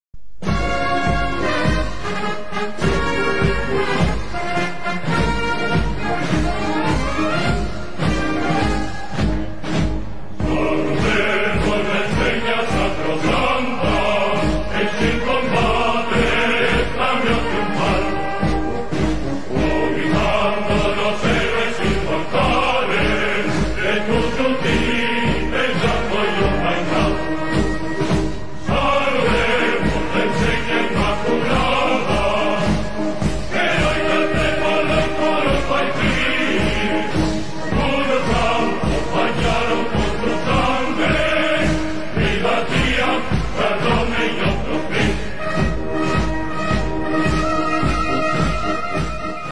XII° Congreso Internacional de Historia de la Guerra de la Triple Alianza
Se trata de un evento internacional en el que participarán historiadores de Argentina, Brasil, Paraguay y Uruguay, que tendrá lugar del 8 al 11 de octubre del corriente año en la Ciudad de Corrientes (Argentina), en el salón del Hotel de Turismo, (Entre Ríos 650).

Organizado por el Instituto de Investigaciones Históricas y Culturales de Corrientes, Subcomisión del Sequicentenario y promovido por el Museo Histórico de Corrientes.